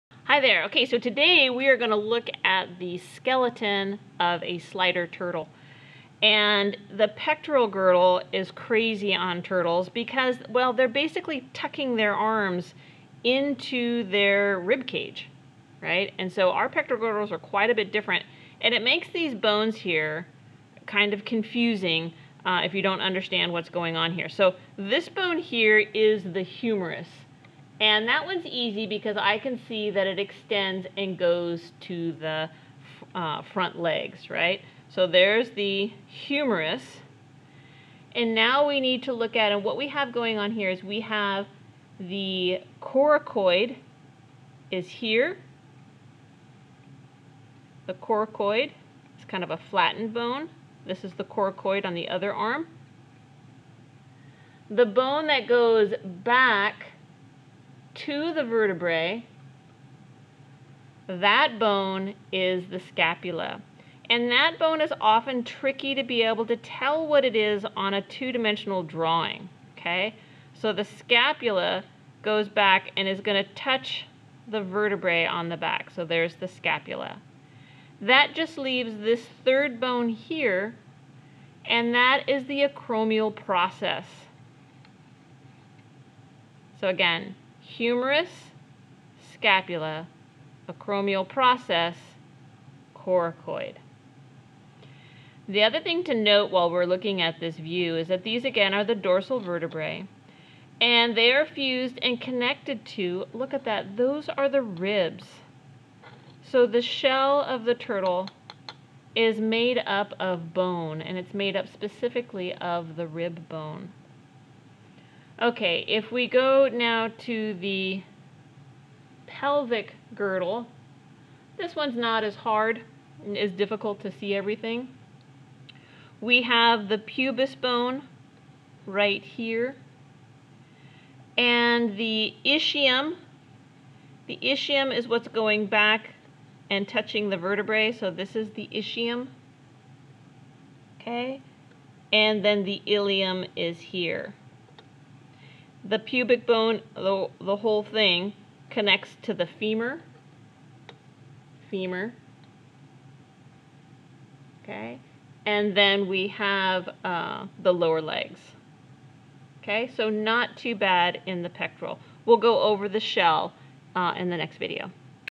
Turtle Skeleton
Anatomy of a turtle skeleton. This is intended for college level biologists.